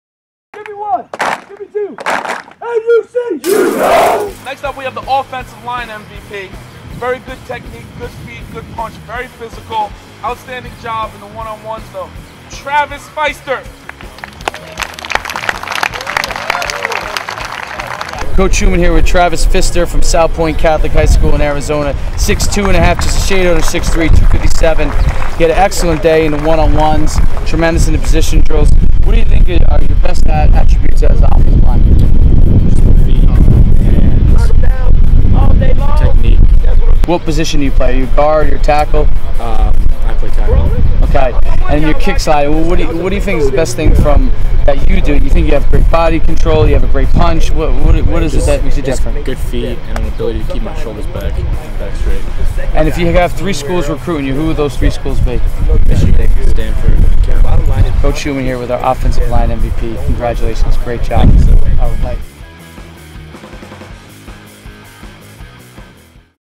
U100 West - Travis Fister - Sophomore Offensive Line MVP - National Underclassmen Combine
Travis Fister let nobody from the DL passed him and went on to capture the OL MVP award at the Ultimate 100 West from National Underclassmen combine